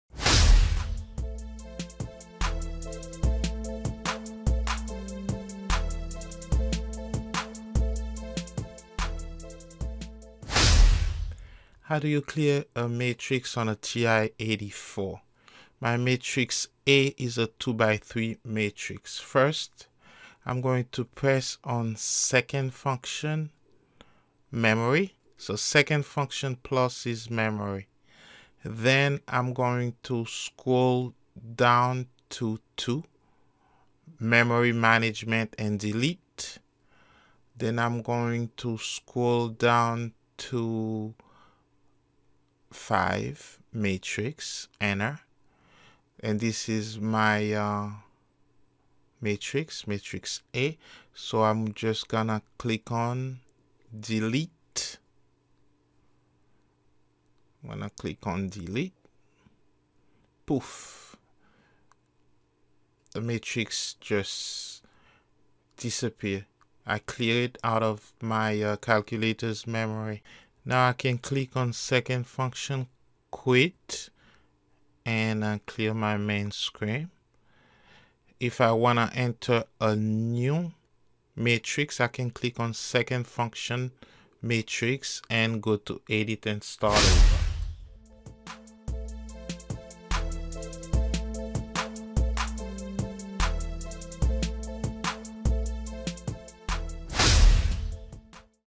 How to delete a Matrix on a Ti 84 or Ti 83 Calculator - Clear Matrix on Ti 83 or Ti 84 calculator
Clear a Matrix on TI 83, Delete a Matrix on a TI 83 or 84